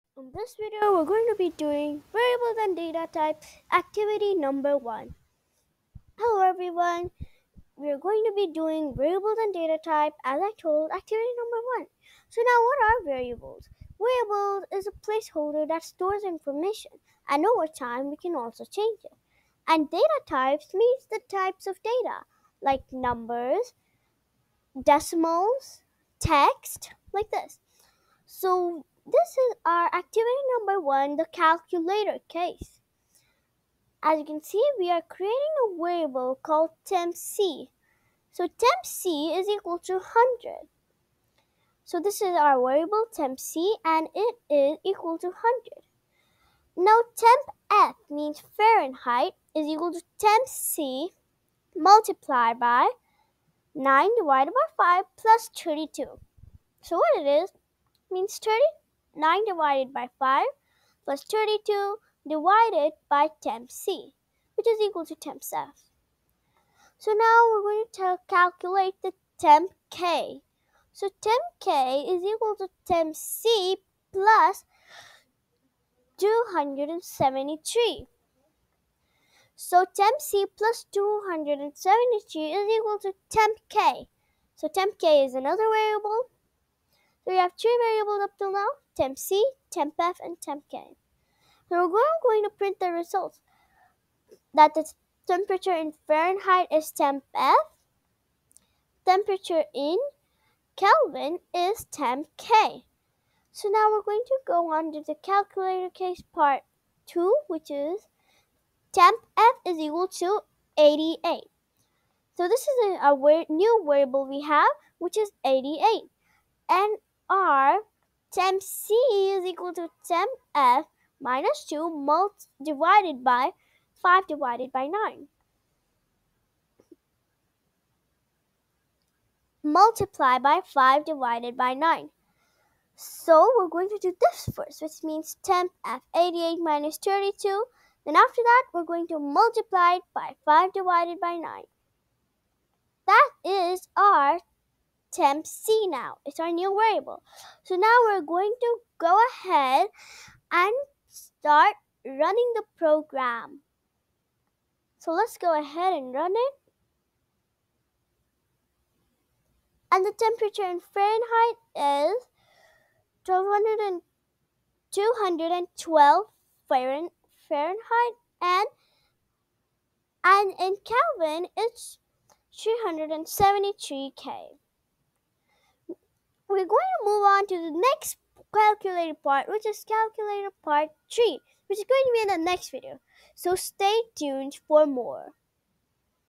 Lesson 5, Variables & Datatypes: Activity no.1
Hello Everyone!
In today's video,
We are going to start turtle programming,
The absolute basics of Python
If you have any doubts,
It will be good for me as well as you!
I'll see you in my upcoming video!!
Bye!